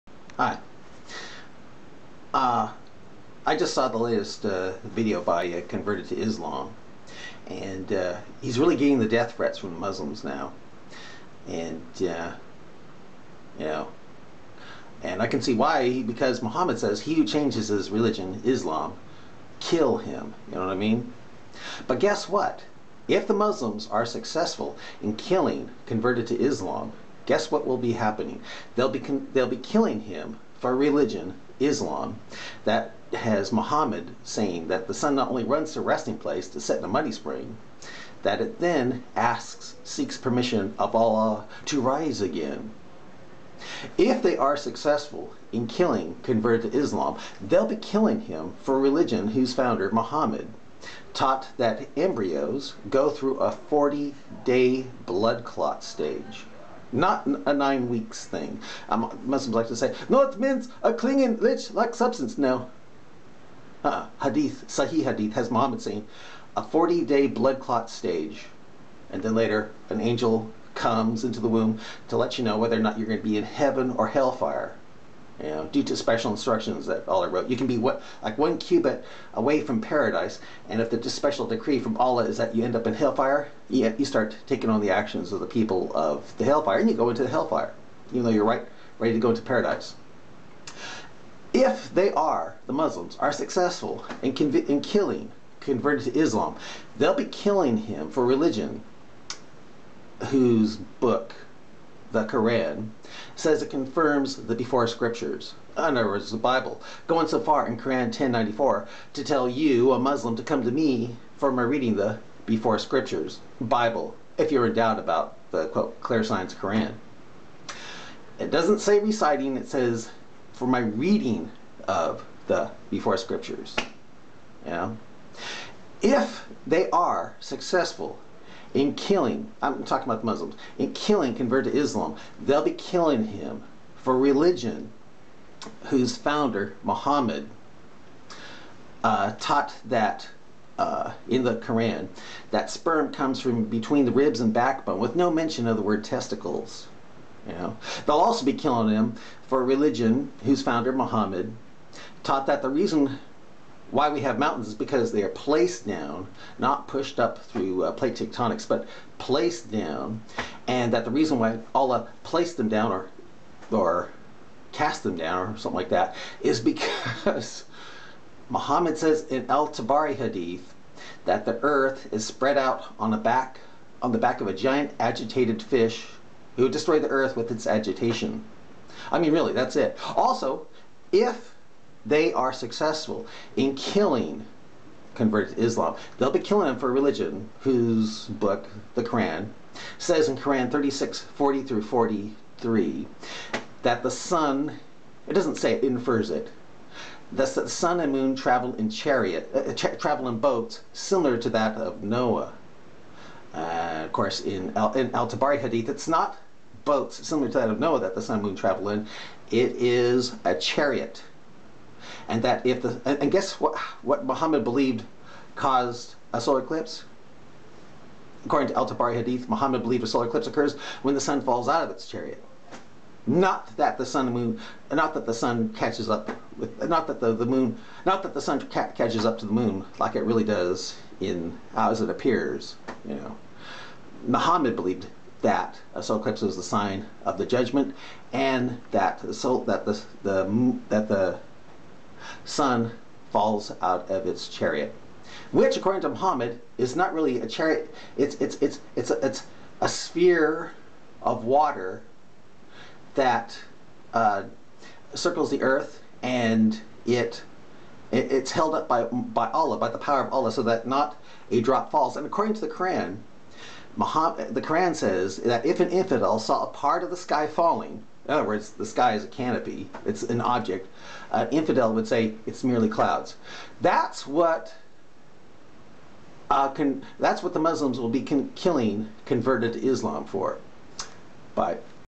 Converted2Islam Getting Death Threats (take 2)
Ex-Muslim, Converted2Islam, is getting death threats from Muslims due to Muhammad saying "he who changes his religion (Islam)... KILL him."  But, WOULD Muslims be so on fire to carry out such an order BY Mr. (pbuh) IF they knew that Muhammad not only taught that the sun "asks/seeks permission (of Allah) to RISE again" (after setting in a MUDDY spring), but, that Quran 36:40-43 infers that the sun and moon "travel in boats similar to that of Noah", which in Al Tabari hadith Muhammad says is a CHARIOT?  WOULD they, the Muslims, be so on fire to KILL "Converted2Islam" IF they knew that the Quran makes NO mention of testicles of where sperm comes from, but, infers that sperm comes "from the ribs and back-bone" and that the bones of a fetus form FIRST?  WOULD they be so on fire to KILL "Converted2Islam" IF they knew that, contrary to what Muslims say about Quran 96 meaning to be "the clinging leech like substance", it really IS "The Clot" thanks to Muhammad saying that en embryo goes through a FORTY day BLOOD clot stage?  DO Muslims know that the Quran not only SAYS it confirms the "before Scriptures" (Bible) ALL Muslims believe to be corrupted, but, in Quran 10:94, tells Muslims to come to people like ME who have READ (not recited) but READ the "before Scriptures" according to Quran 10:94 IF they, the Muslims, are in doubt about the "clear signs" (Quran)?  WOULD Muslims try to KILL "Converted2Islam" IF they knew that the Quran taught that the REASON we have mountains is NOT because of moving tectonic plates, but, because mountains are PLACED down, by Allah, to keep the earth from being destroyed DUE to being "spread out" on the back of a giant fish called "Nun" according to Muhammad in Al Tabari hadith?  There is more, but, I'll have to later put it in comment form instead of video
description.  I wonder if Muslims would still want to kill "Converted2Islam" now if they just knew about THOSE things taught in Islam.